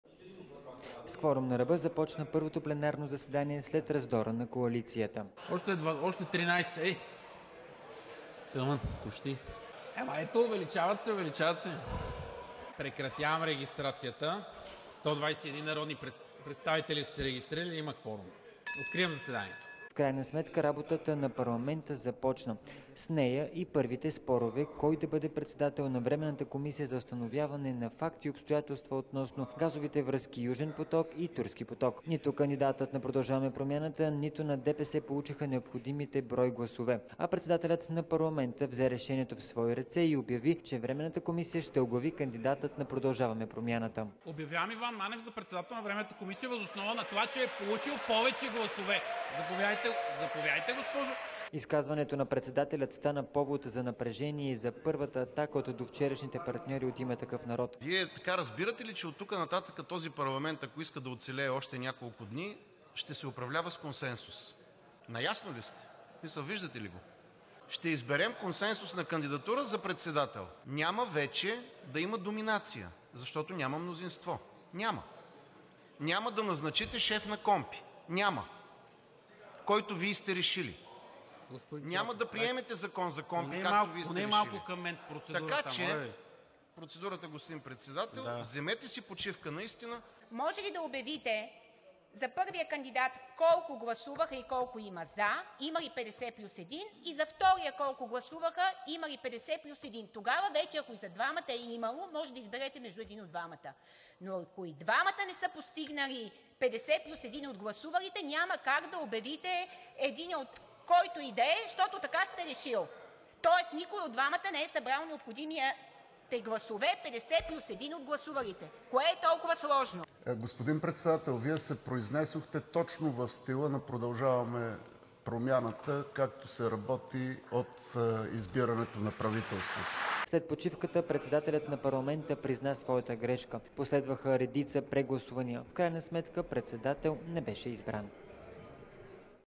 Кворум на ръба и спор в НС след труса: Патова ситуация за "Южен поток" и "Турски поток"
Искрите в управляващата коалиция и крехкото мнозинство се видяха още в първото заседание. В крайна сметка работата на парламента започна. С нея и първите спорове - кой да бъде председател на Временната комисия за установяване на факти и обстоятелства относно газовите връзки "Южен поток" и "Турски поток".